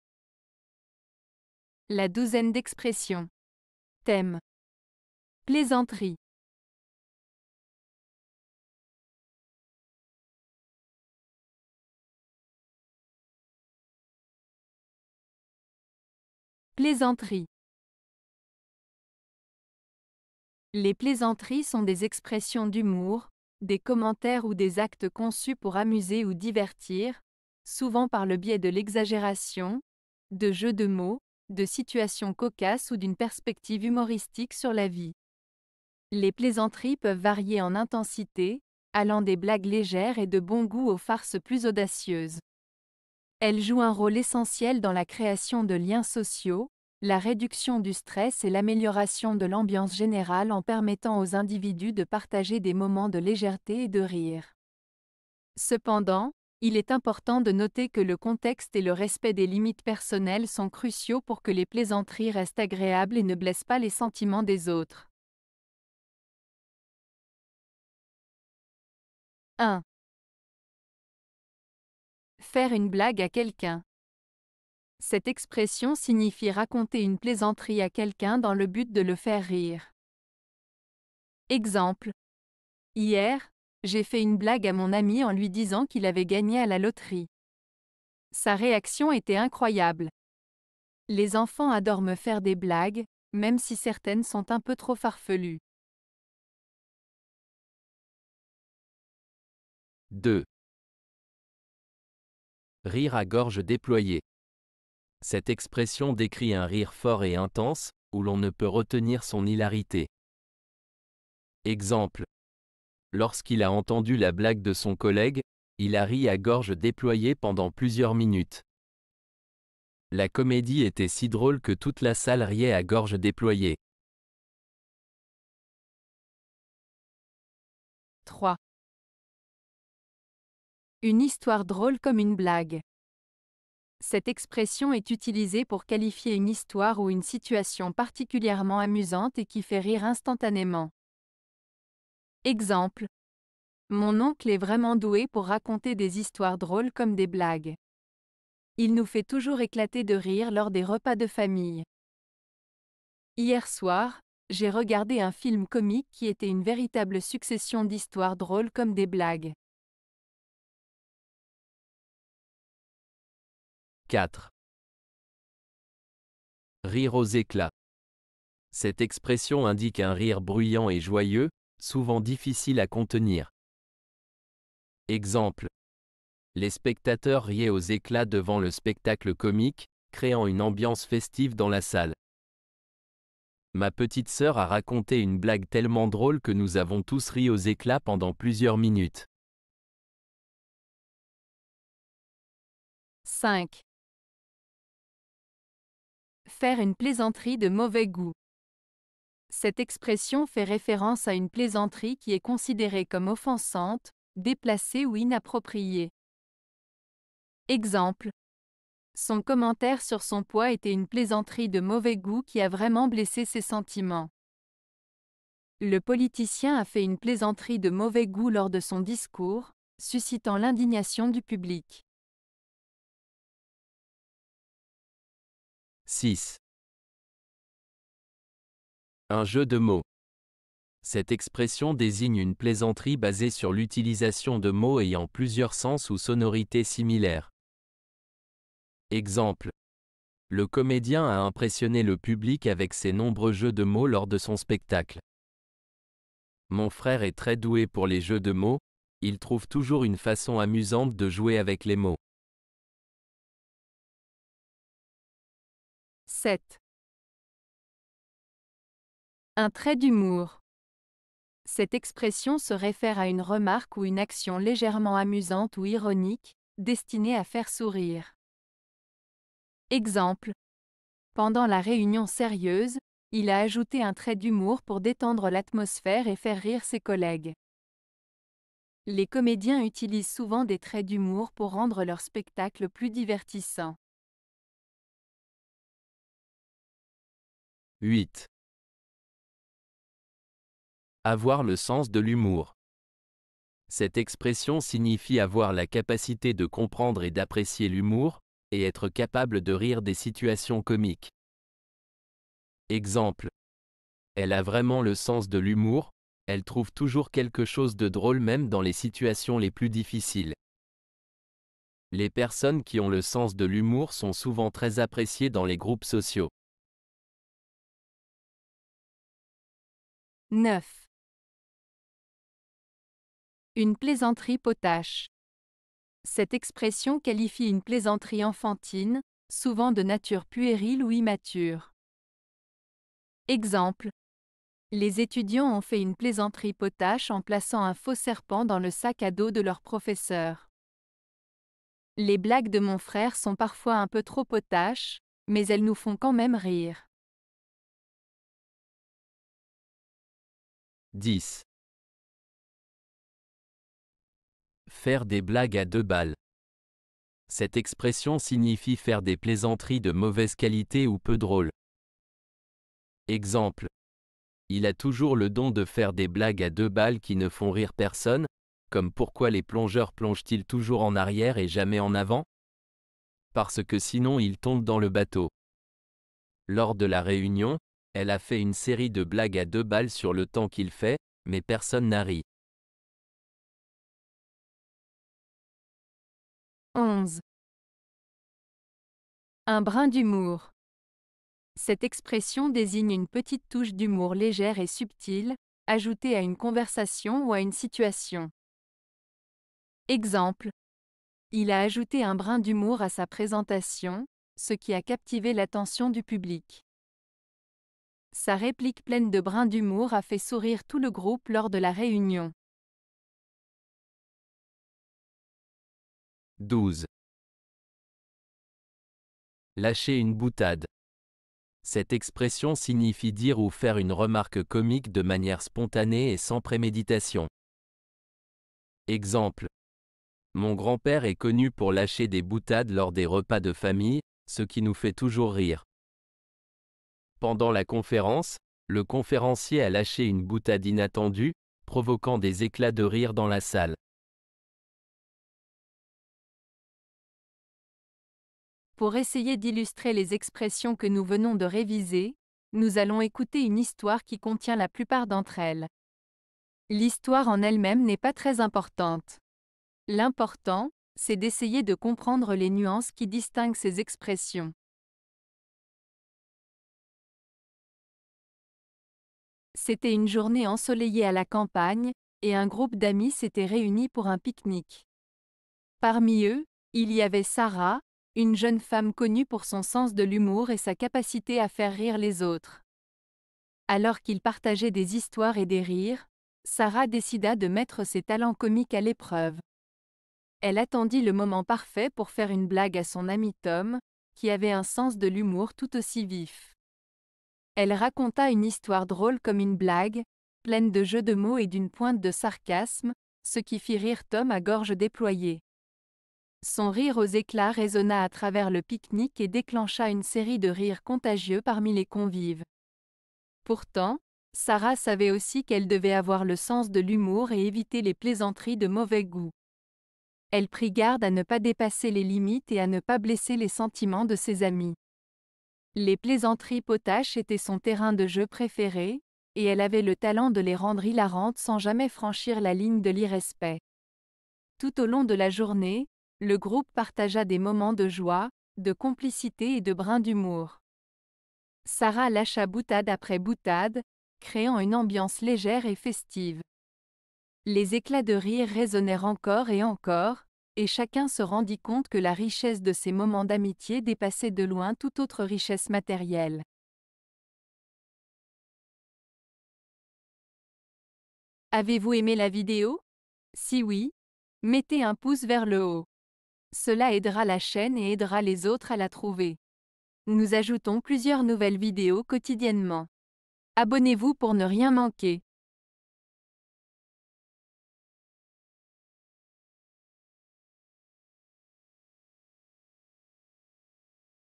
Thème - Plaisanteries (La douzaine d'expressions)
00:00 Introduction
00:18 Définition: Plaisanteries
01:10 1. Faire une blague à quelqu'un
01:41 2. Rire à gorge déployée
02:09 3. Une histoire drôle comme une blague
02:45 4. Rire aux éclats
03:15 5. Faire une plaisanterie de mauvais goût
03:47 6. Un jeu de mots
04:18 7. Un trait d'humour
04:49 8. Avoir le sens de l'humour
05:22 9. Une plaisanterie potache
05:52 10. Faire des blagues à deux balles
06:28 11. Un brin d'humour
06:58 12. Lâcher une boutade
07:47 Histoire